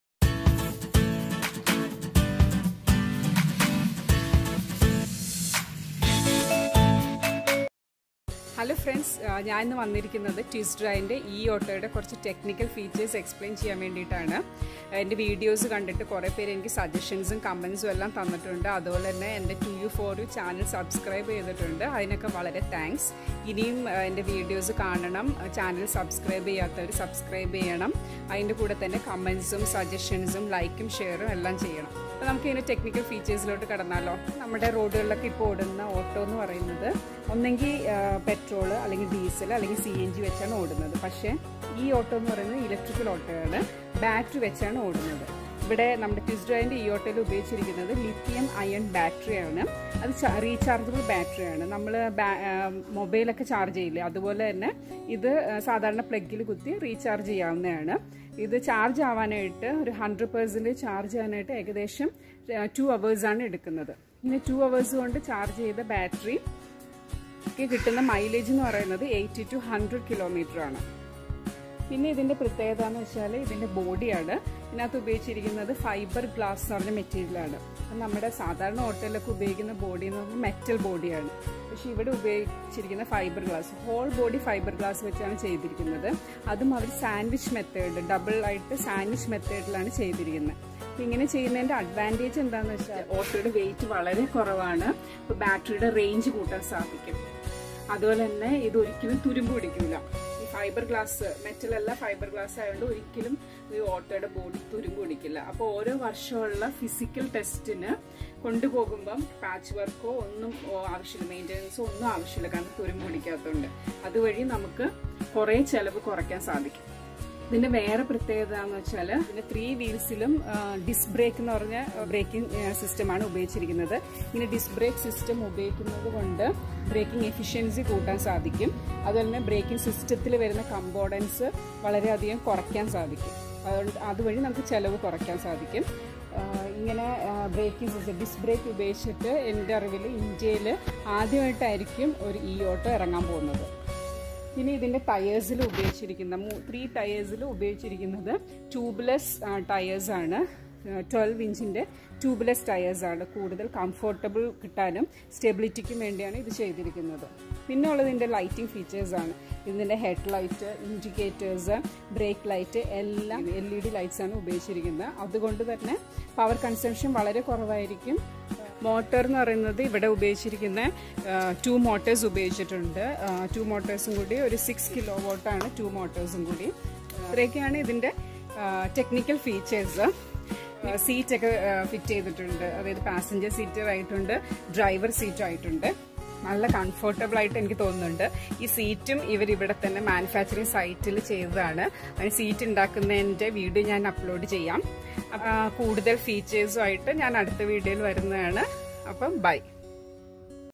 ഇലക്ട്രിക്ക്ഓട്ടോ electric auto rickshaw/TystDriveEauto/Technicalfeatures/Fiberglass@Lithiumion
Vlog05: Tech
Tyst Drive E auto Technical Features@ Fully fiber glass body@Lithiumion battery@Discbreak system@LED lighting@Tubeless tyres # 100% pure electric 0% emission @ go green. India's first launching E auto with all these features